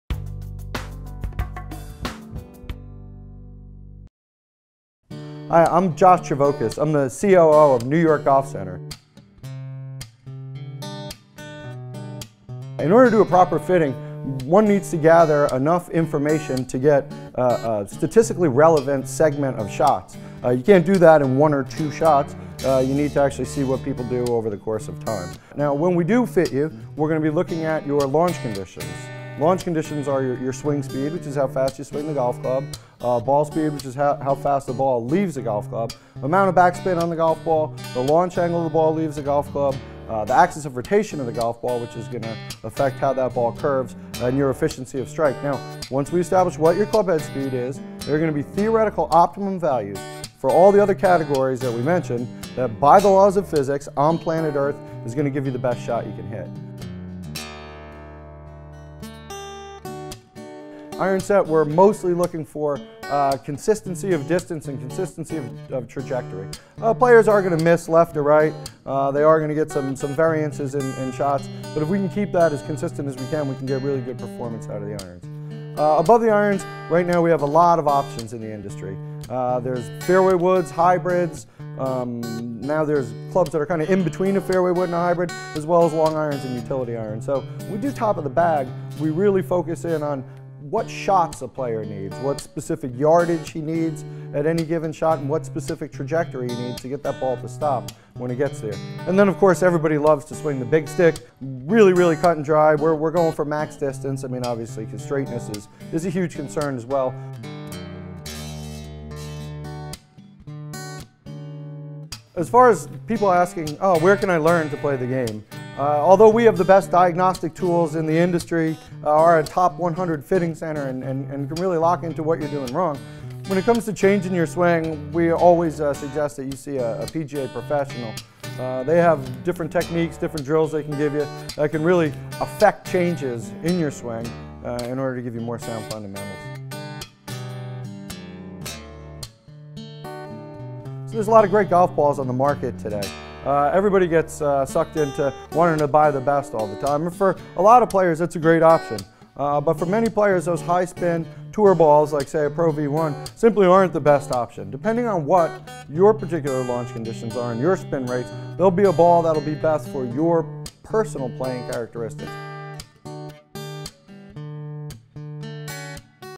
How To Pick The Right Golf Clubs
Here are some tips on how you can find the right golf clubs.